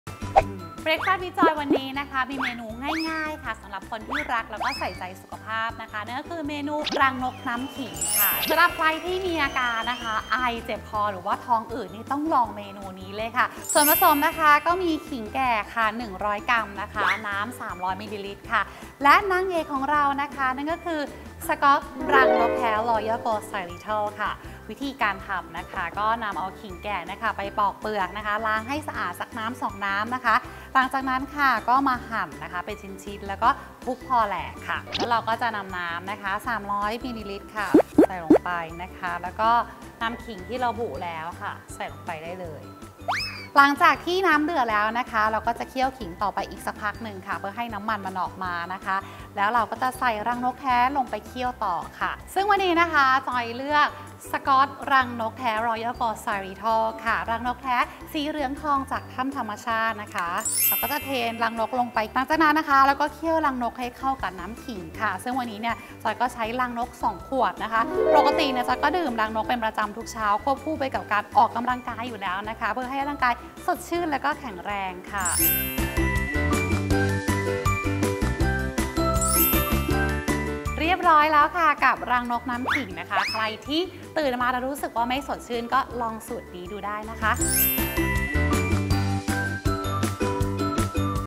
รังนกน้ำขิง l Breakfast with Joy
เมนูง่ายๆ ยามเช้าที่มีประโยชน์ต่อสุขภาพ

1. ขิงแก่ 100 กรัม
2. น้ำ 300 มิลลิลิตร
3. รังนก 2 ขวด

- ขิงแก่มาปลอกเปลือก จากนั้นล้างน้ำให้สะอาด 1-2 น้ำ
- หั่นขิงเป็นชิ้นๆ แล้วทุบพอแหลก
- ใส่น้ำลงในหม้อ พร้อมใส่ขิงที่ทุบพอแหลกลงไป
- หลังจากที่น้ำเดือดแล้วเคี่ยวต่ออีกสักพักเพื่อให้น้ำมันออกมา
- ใส่รังนกลงไป 2 ขวด เคี่ยวให้เข้ากับน้ำขิง
- เมื่อเคี่ยวจนได้ที่ ตักใส่ชามพร้อมเสิร์ฟ

ติดตามเปรี้ยวปากผ่านช่องทางอื่นๆได้ที่